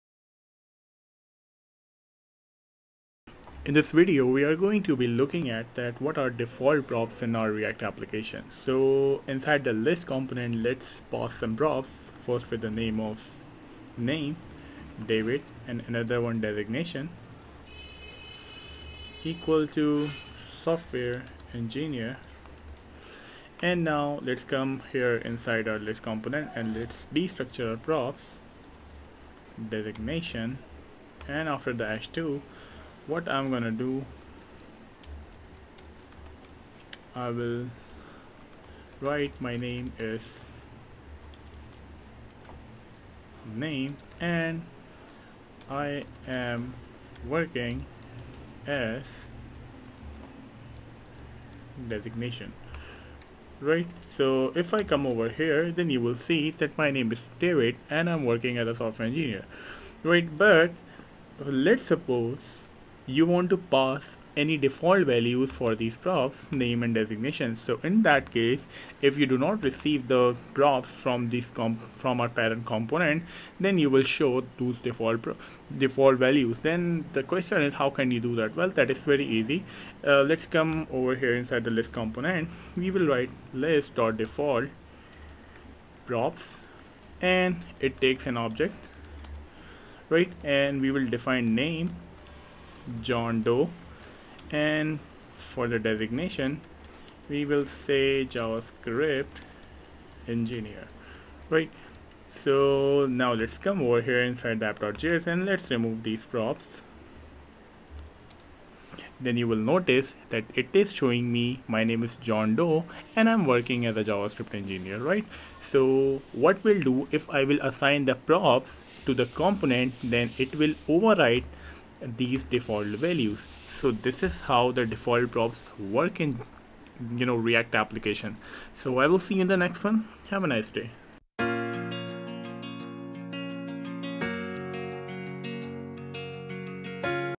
React JS Tutorials | DefaultProps in React | React JS From Beginner to Advanced Crash Course
Series: React JS Tutorials | React Crash Course

In this video we will learn that what are DefaultProps in React JS, and how can add the default values to our props in React JS.